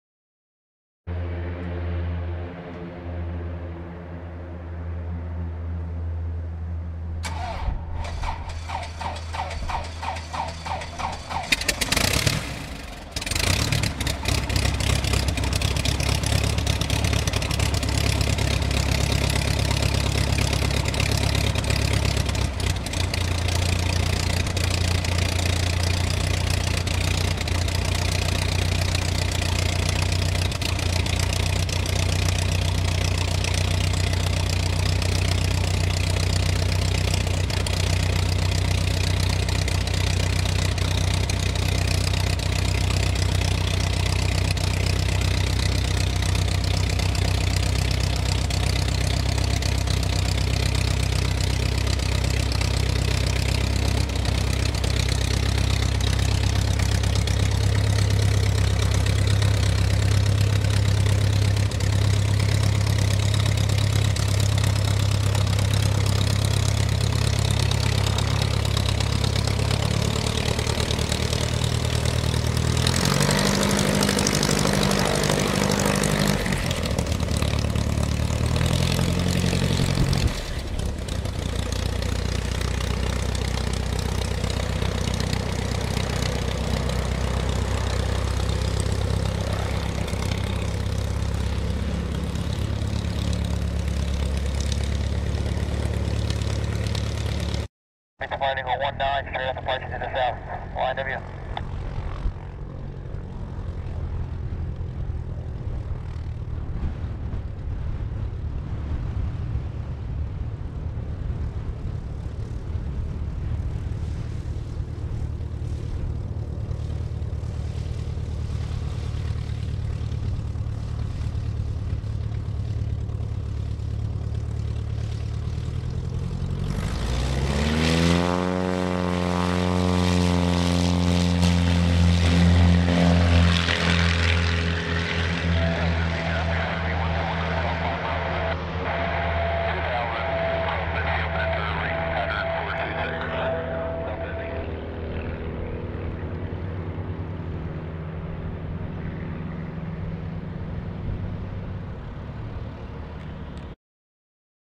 CHRISTEN EAGLE II
The Christen Eagle II, which later became the Aviat Eagle II in the mid-1990s, is an aerobatic sporting biplane aircraft that has been produced in the United States since the late 1970s.  Designed to compete with the Pitts Special by Frank Christensen originally of Salt Lake City, Utah, the Eagle II is marketed in kit form for homebuilding. The Eagle II is a small aircraft of conventional configuration with single-bay, equal-span staggered biplane wings braced with streamlined flying and landing wires and an I-strut to form a box truss. The pilot and a single passenger sit in tandem underneath a large bubble canopy. The tailwheel undercarriage is fixed, with the mainwheels mounted on spring aluminum legs. The main wheels are housed in streamlined fairings. The fuselage and tail are constructed of chromoly steel welded tube, with the forward fuselage skinned in aluminum and the rear fuselage and tail covered in fabric. The wing structure is Sitka spruce wood and fabric covered. The engine cowling is fiberglass.

This Christen Eagle II is seen starting up and departing Runway 19 from the Flying W Airport for some aerobatic instruction.

Please visit the ZINGER AVIATION MEDIA STORE for merchandise and to support ZINGER AVIATION MEDIA.